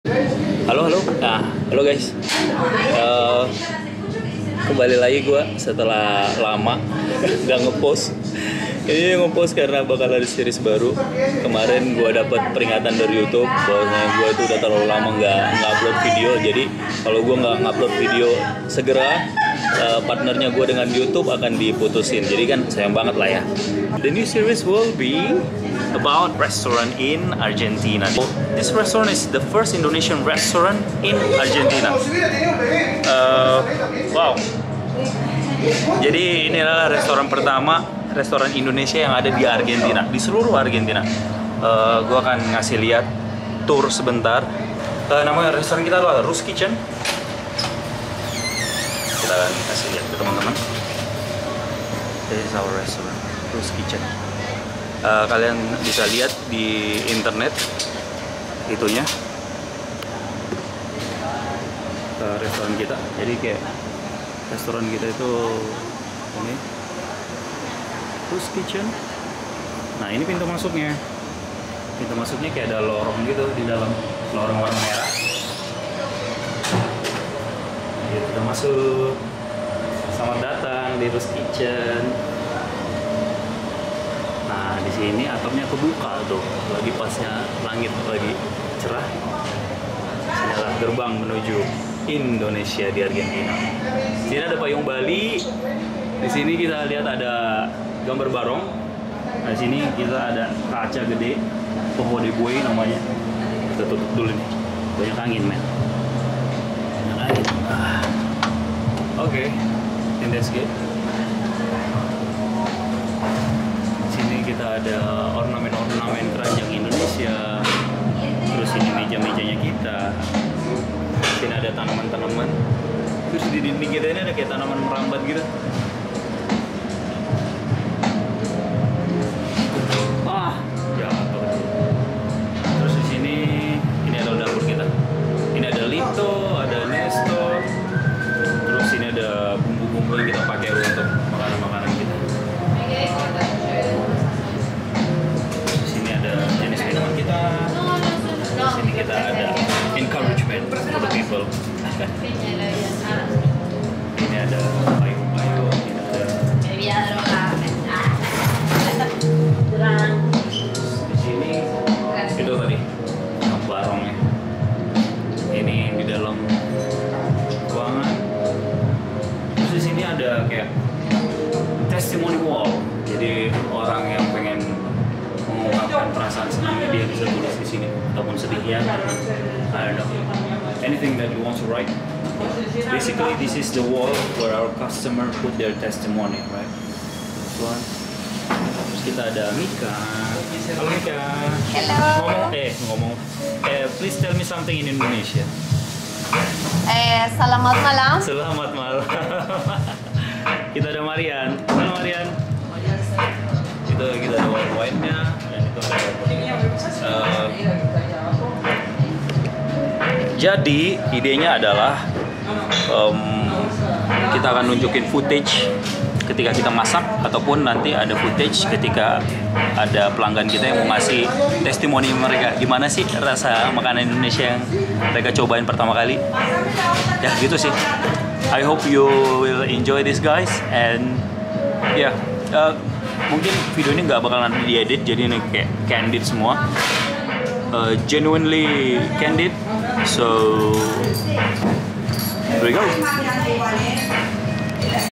SATU-SATUNYA restoran INDONESIA di ARGENTINA. Ru's Kitchen. Ep. 1
Share and like the video is a huge support for us.

TERIMA KASIH